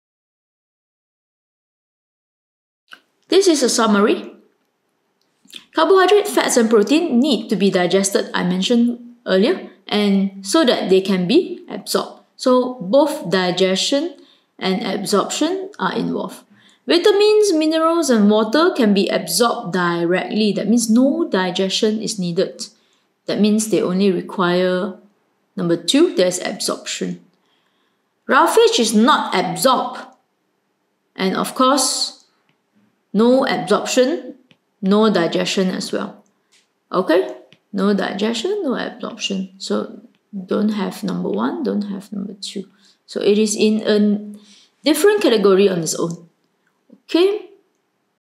Digestion and absorption of nutrients in food
Carbohydrates, fats and proteins need to be digested before absorption.
Vitamins, water and mineral salts need not be digested and are absorbed directly.
Dietary fibre is not absorbed.